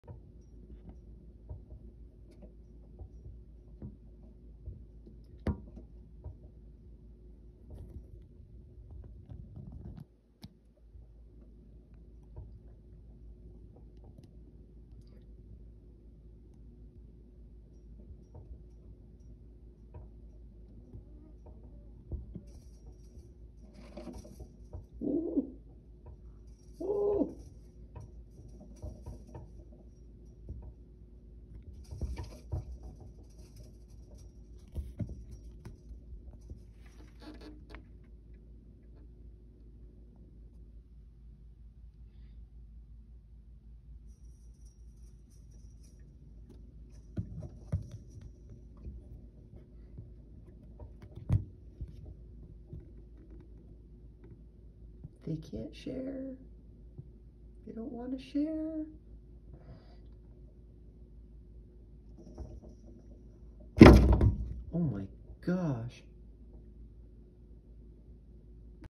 Crazy Squirrels At The Feeder!
The squirrel at the very end jumps towards me and HITS THE WINDOW!!! It scared me lol...